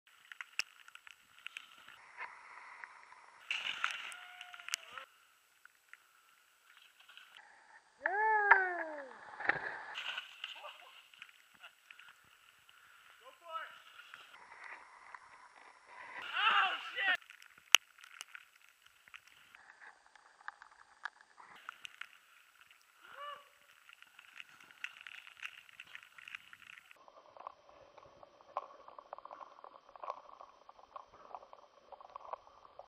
A short from Stevens Pass - January 8th, 2011
We steeped off trail a bit to have some fun over a cat track.